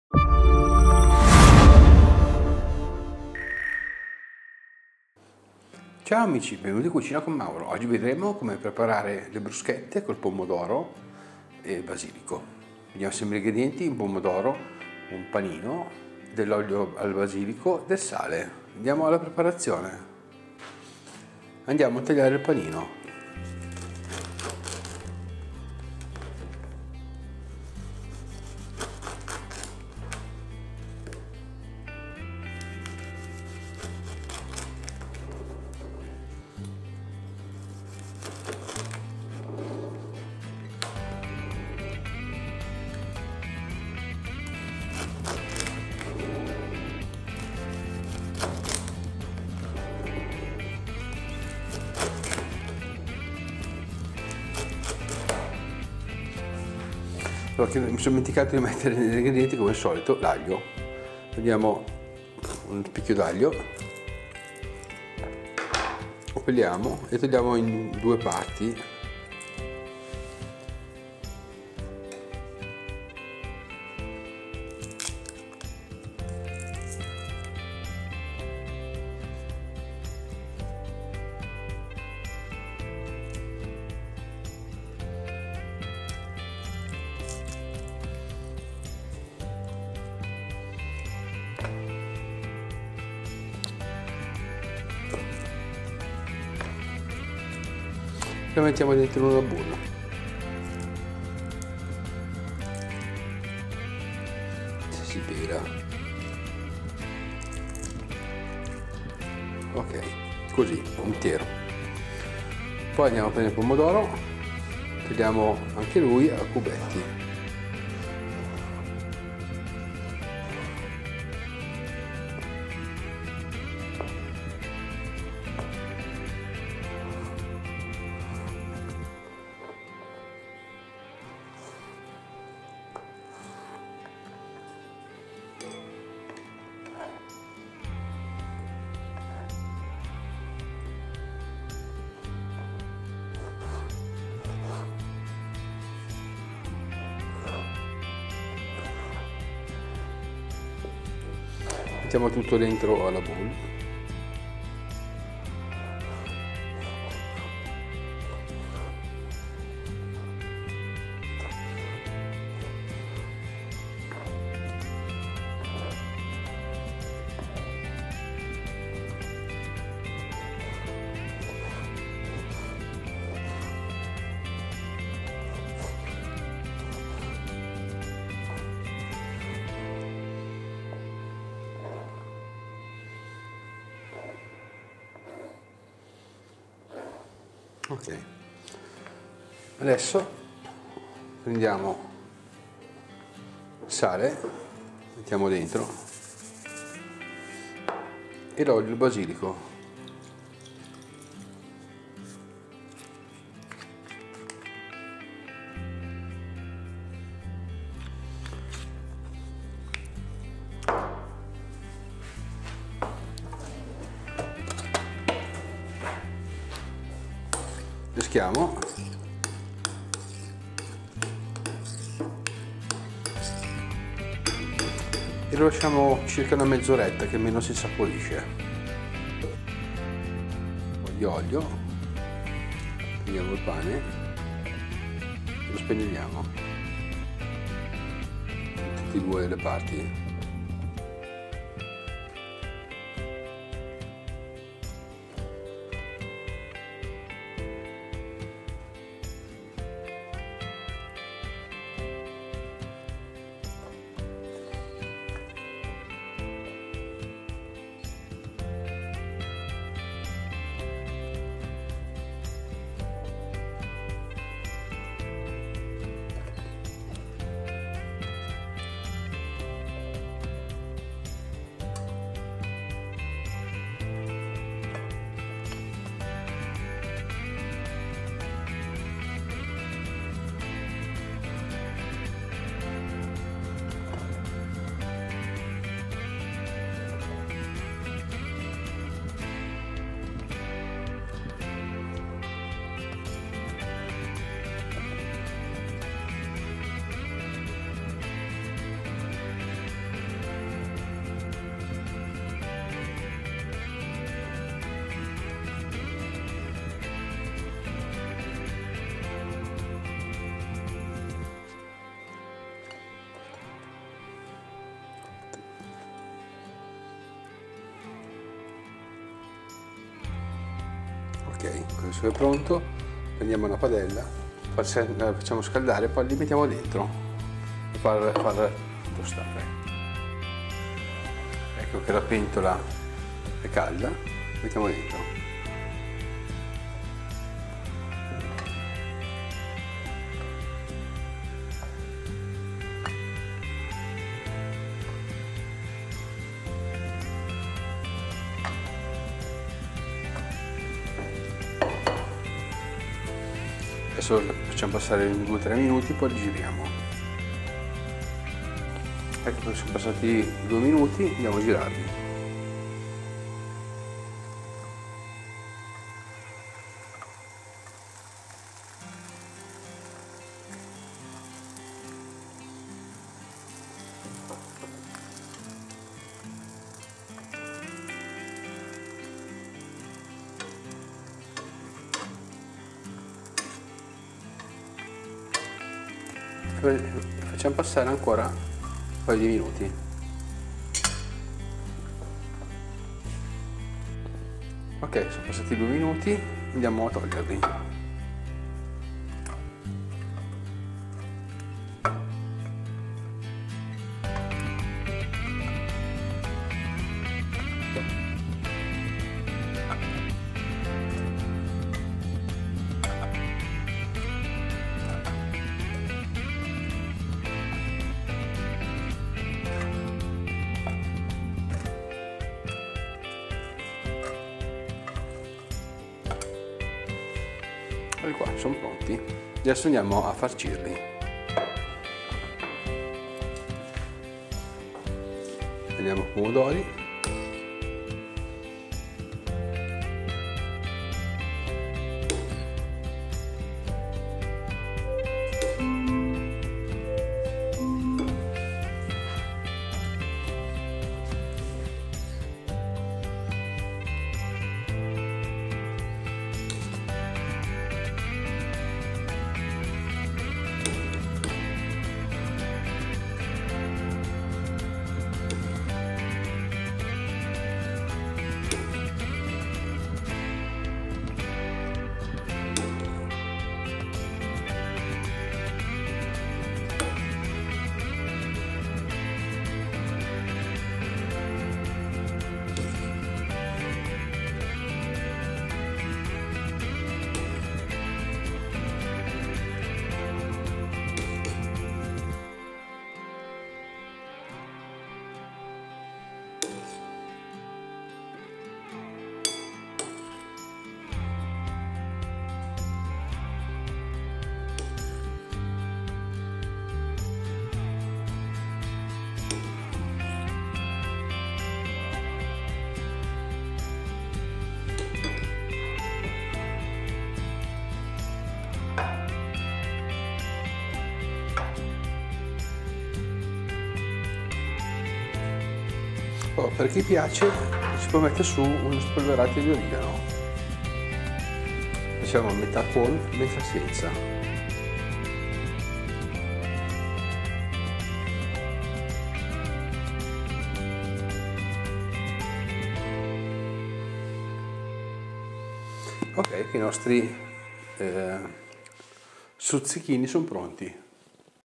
crostini facili e deliziosi
ingredienti 1 panino ciabatta
1 pomodoro
olio al basilico
sale
aglio
olio normale